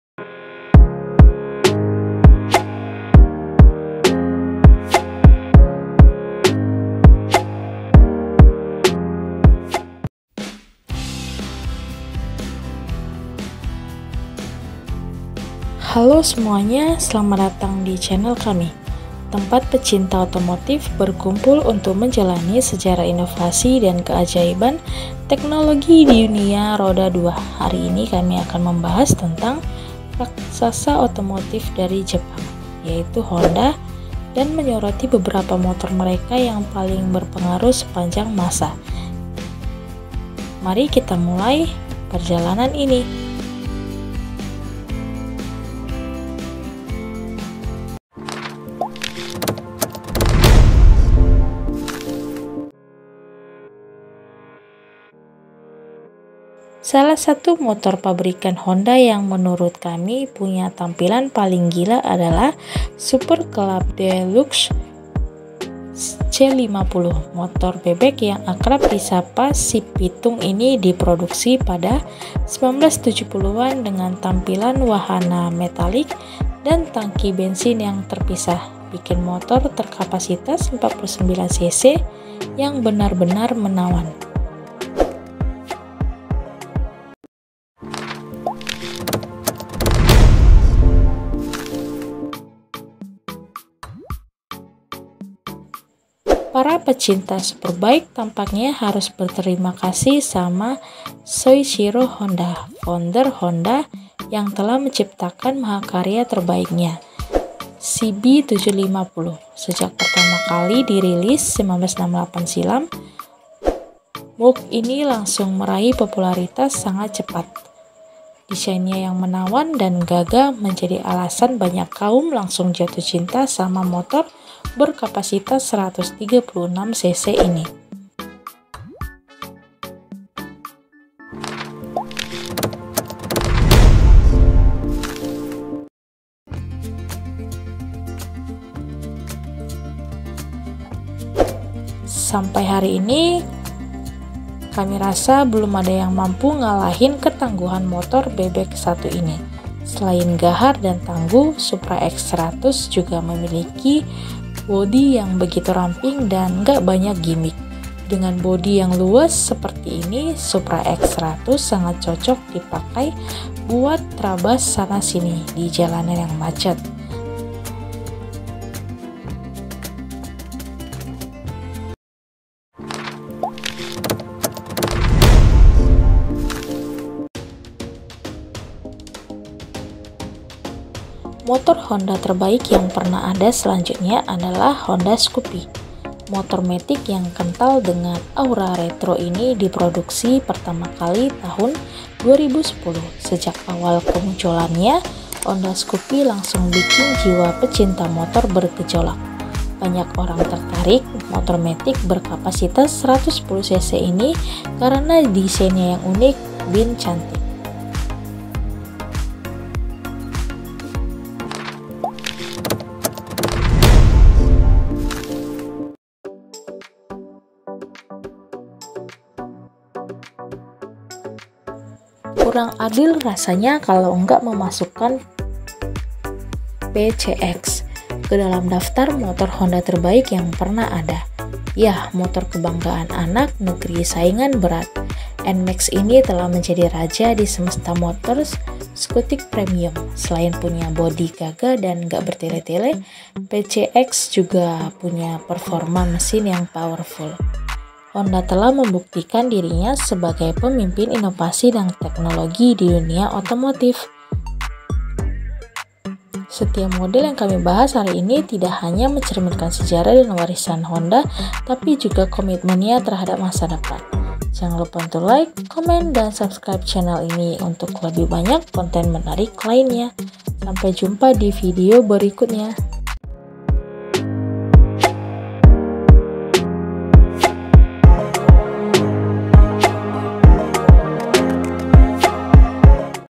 Motor Honda Paling Berpengaruh Di Dunia Otomotif Sepanjang Masa - TOP5
Honda, merek yang telah menjadi sinonim dengan inovasi dan keandalan dalam industri otomotif, tidak hanya dikenal karena mobilnya yang efisien dan tahan lama tetapi juga karena sepeda motornya yang revolusioner.

Silakan juga kunjungi website kami, untuk info berita terkini dan paling menarik.

Creator : MT